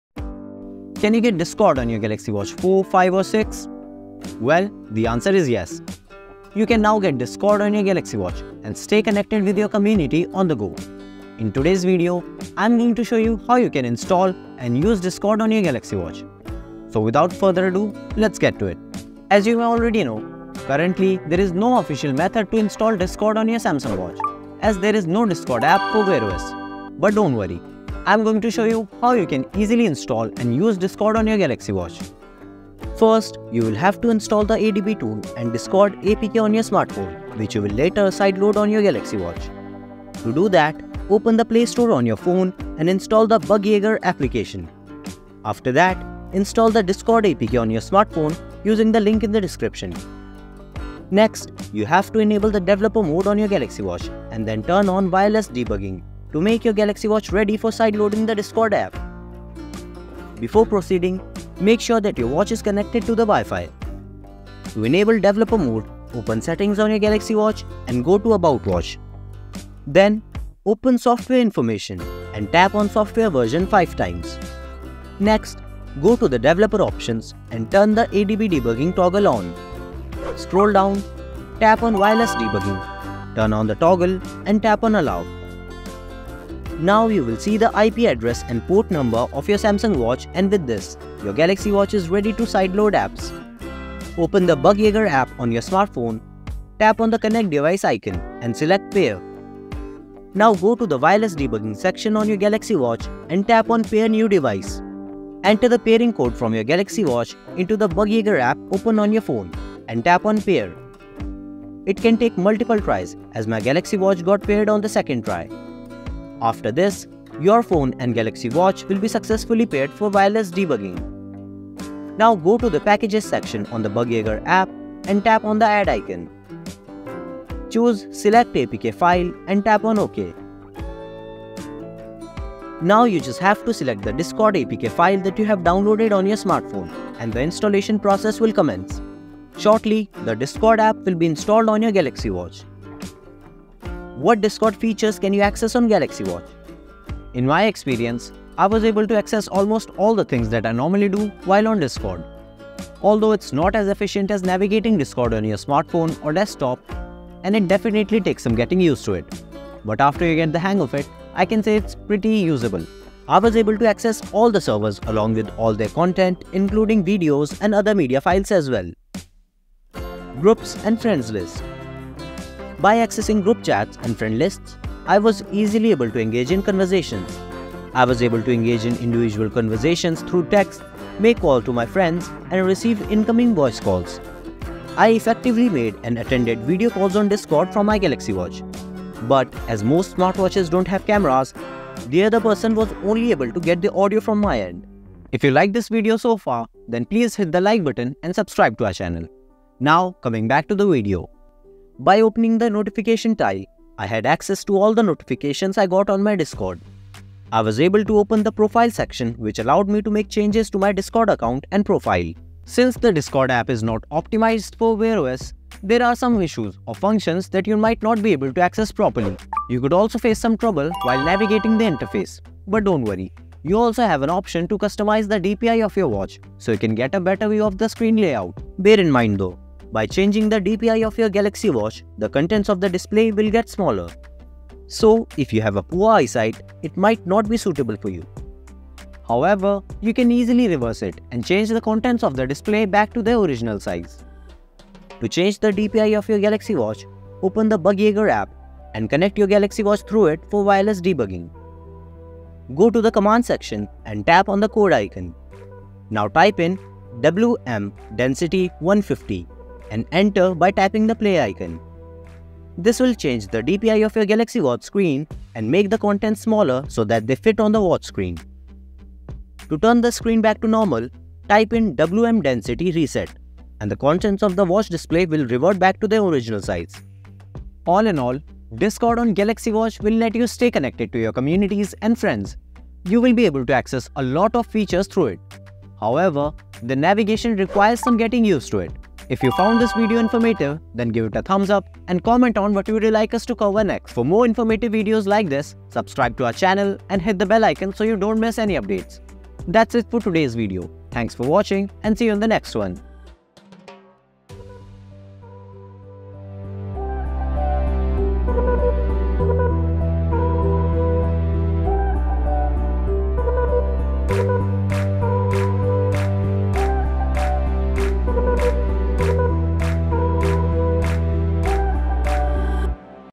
How To Get Discord On Samsung Galaxy Watch 6, 5, 4? Check Discord Notifications On The Go
Learn how to get Discord notifications on Samsung Galaxy Watch! Whether it's Galaxy Watch 4, 5, or 6, this simple tutorial will show you how to set up and use Discord on your Samsung watch. Watch now for step-by-step instructions.